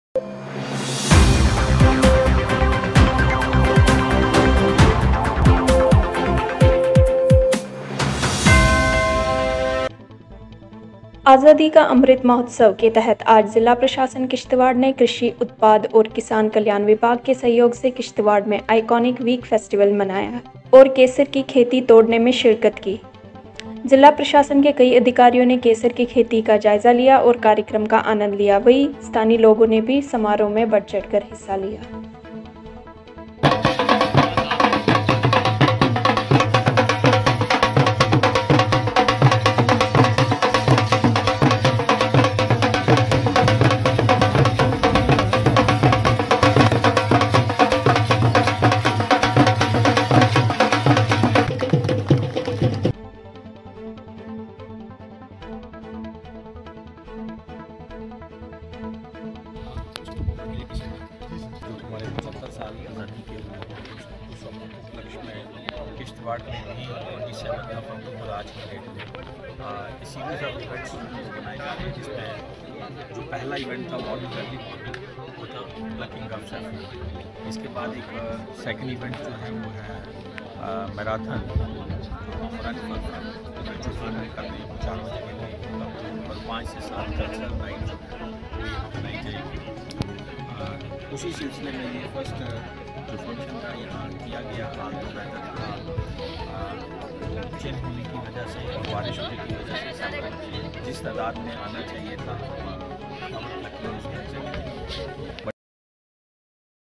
District Administration Kishtwar celebrated Saffron Plucking Festival
District Administration Kishtwar in collaboration with Dept of Agriculture Production & Farmers Welfare Kishtwar celebrated Saffron Plucking Festival at Pochhal, Kishtwar under Iconic Week celebration